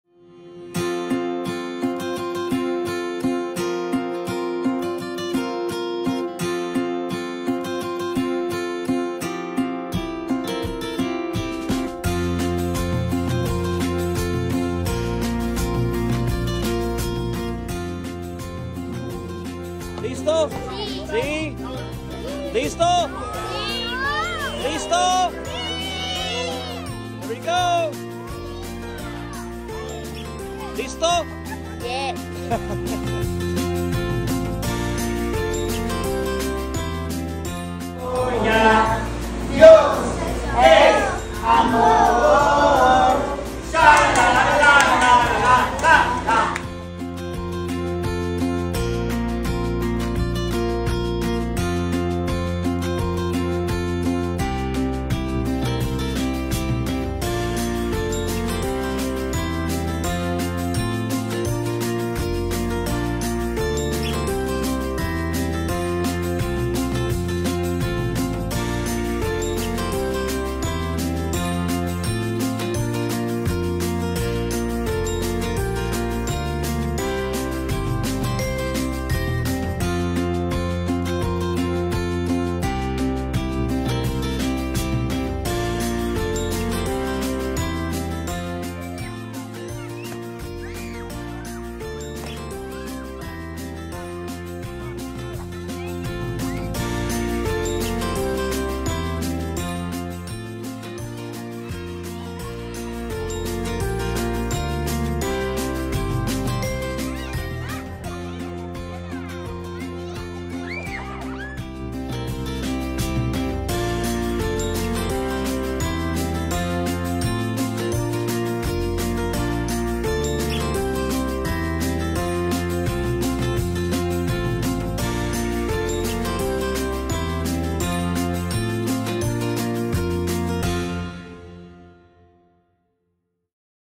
2023 VBS Missions Trip to Mexico
Our Coronado missions team traveled to San Ignacio to help ​⁠@HandsAcrossBorders and their 2023 Vacation Bible School. We were able to see 21 souls come to Christ for salvation! To God be the glory!